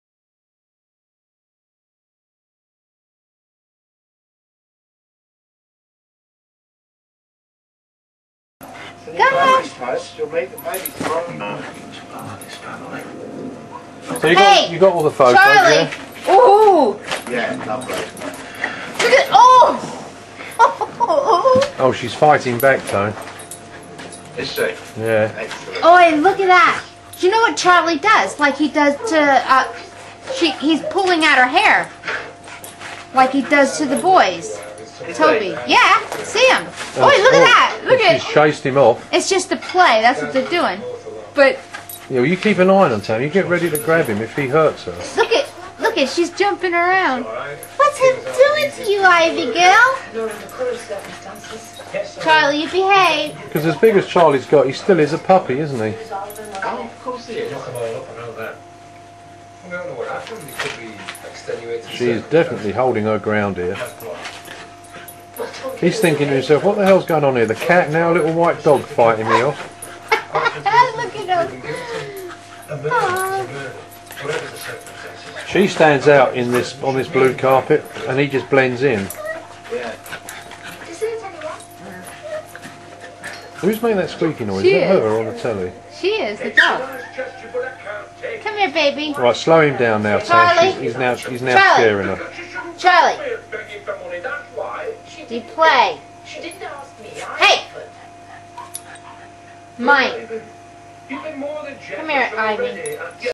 Ivy's first night
This is Ivy our new puppy born 1st Feb 2007. She is showing Charlie she is going to be boss.